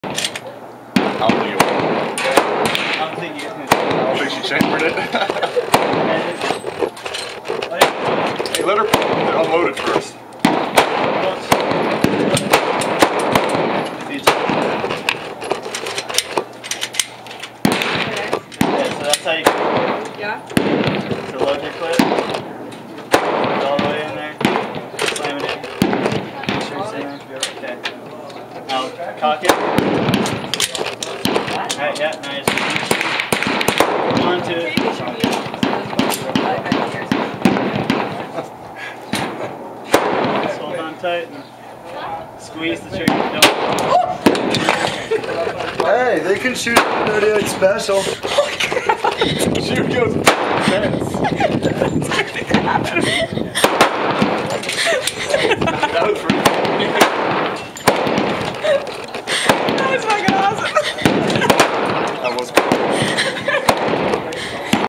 Shootin' Guns in Arizona
Alex shooting her first gun in Arizona